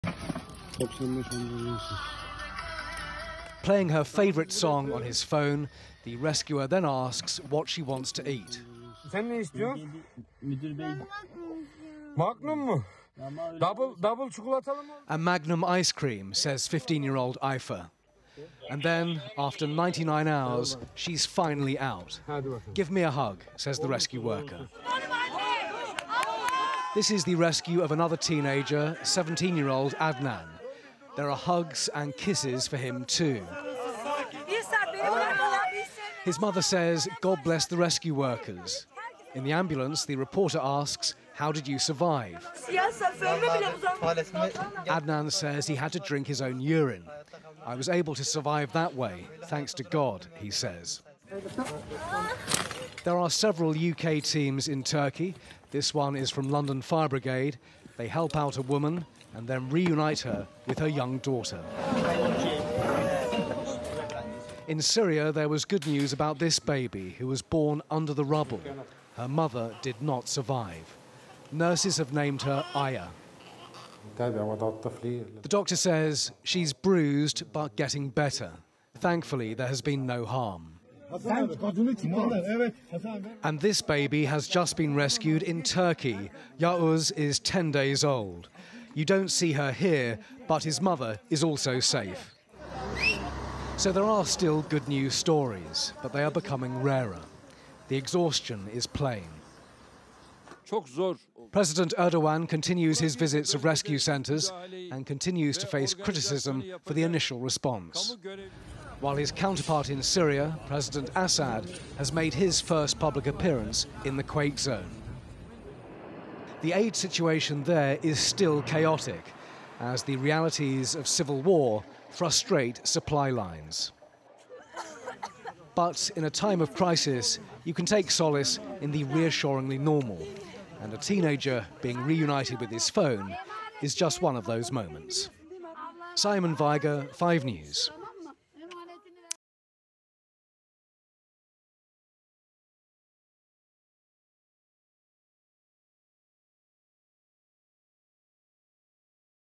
Turkey and Syria death toll continues to rise | 5 News
►All this week, we've shown loss and destruction on an almost unimaginable scale after earthquakes devastated huge regions of southern Turkey and Northern Syria in the early hours of Monday morning.